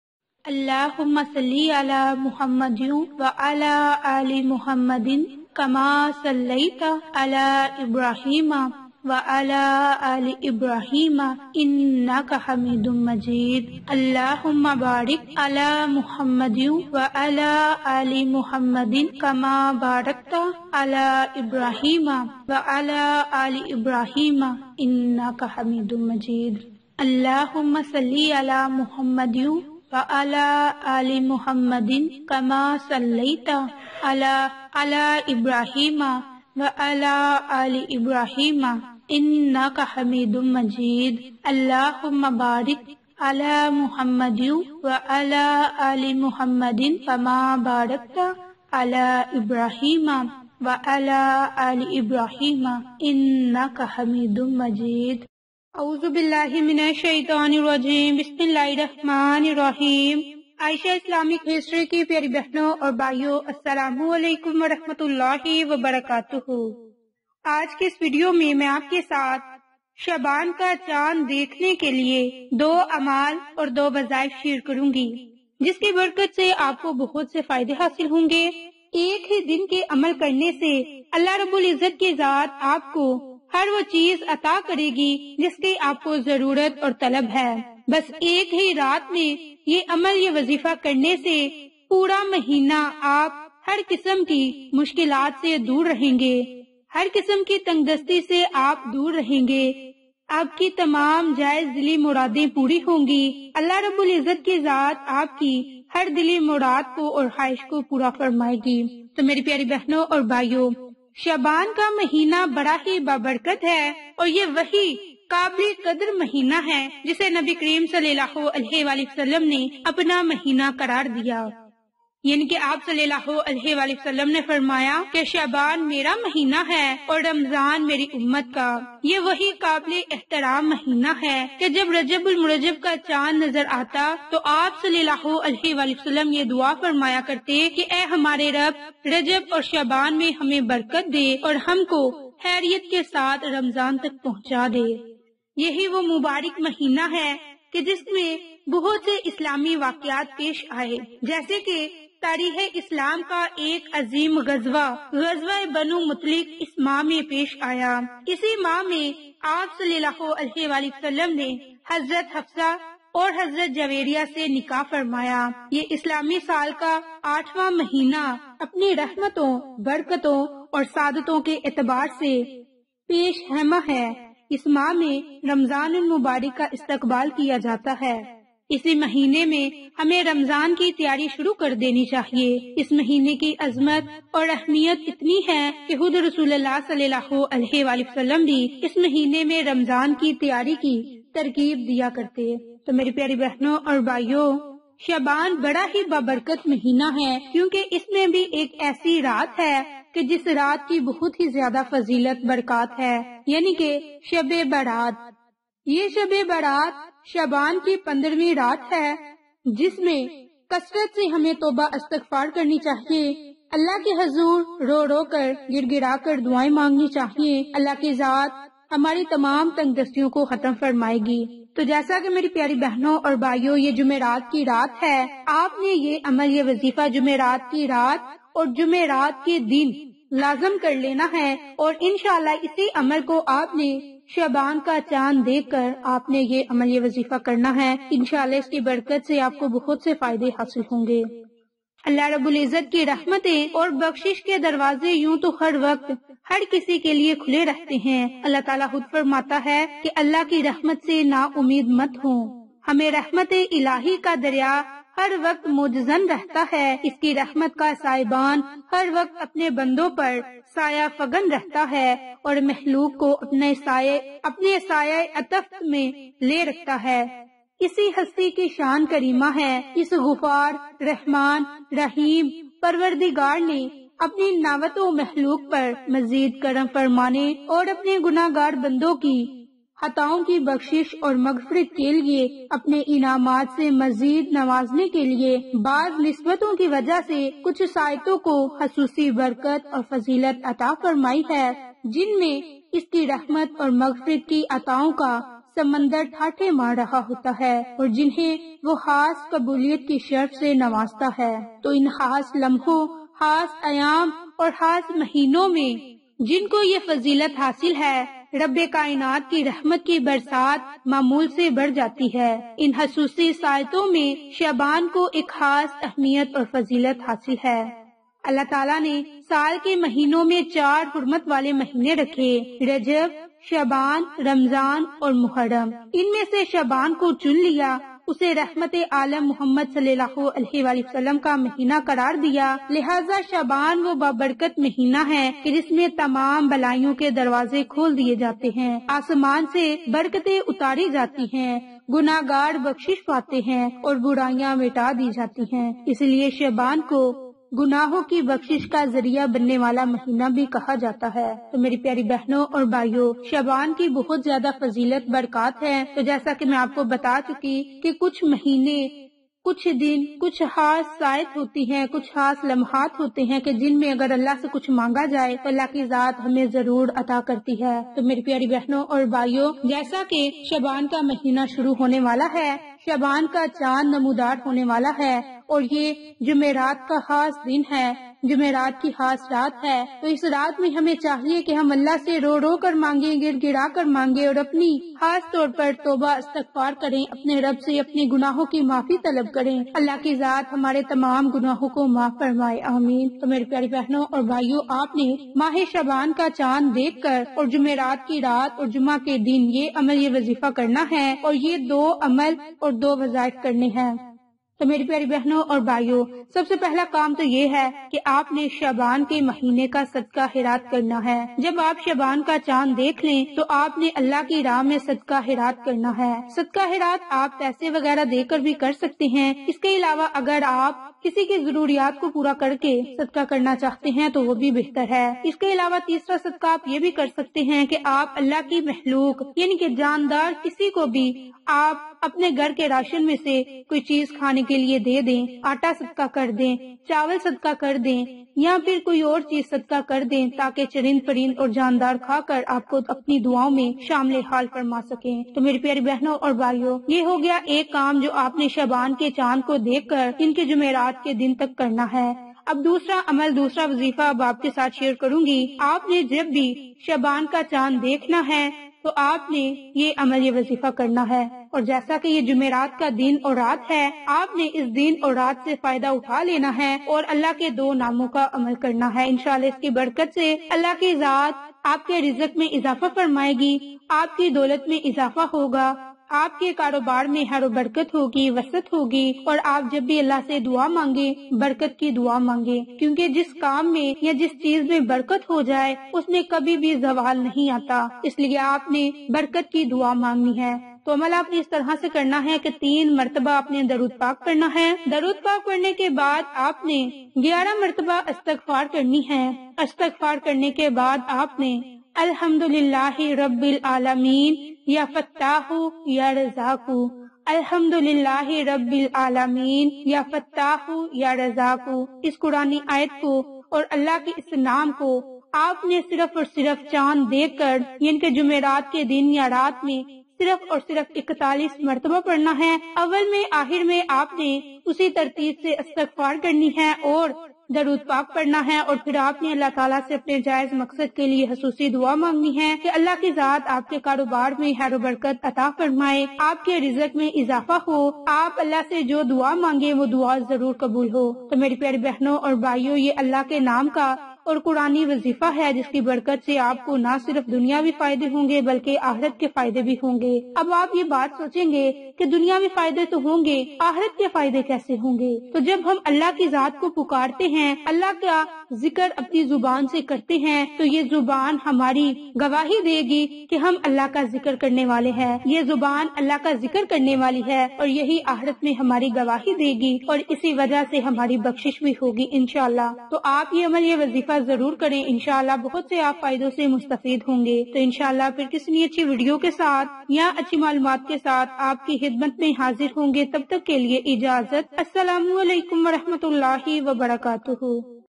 Rajab ki akhri jumerat ke din Allah k naam ka wazifa for hajat | Shaban ki fazilat | Jumerat ka amal
Aj mein ap ke sath Rajab ki akhri jumerat ke din Allah k naam ka wazifa for hajat | Shaban ki fazilat | Jumerat ka amal, yeh amal ap rajab ki last jumerat ke din lazim kry is ke ilawa ap yeh amal or wazifa shaban ka chand dekh kar lazim kar ly InshaAllah apki har hajat puri hogi har dua qabool hogi